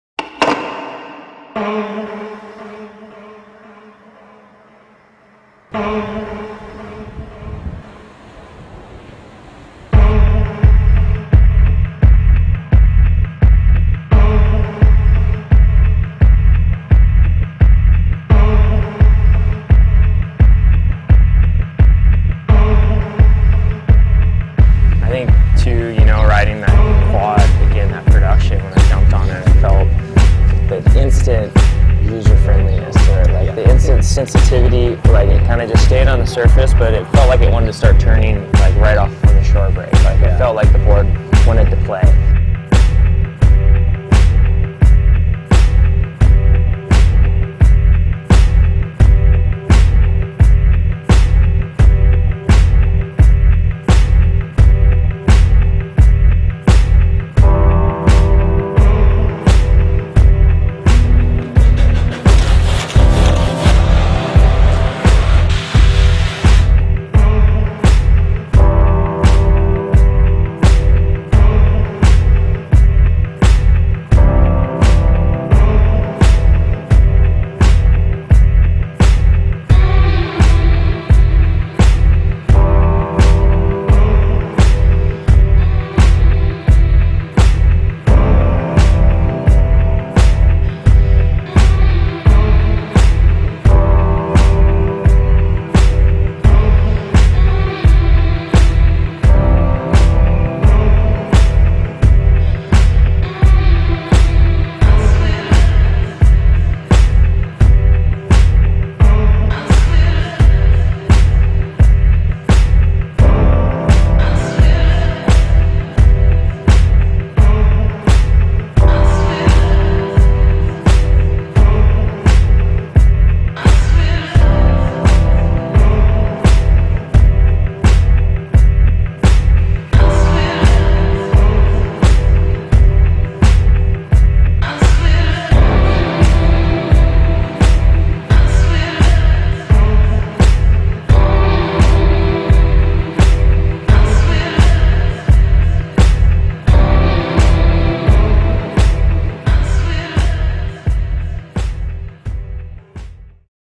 The LS Quad and Thruster.
Quatro Windsurfing boards and Levi Siver have had a significant impact on the world of windsurfing, particularly in the realm of wave-riding.
Yet, one of Quatro's most remarkable legacies lies in the unwavering loyalty of numerous professional riders who, throughout the years, have opted to ride Quatro boards even when they were cleverly sprayed to resemble their official sponsor's brands.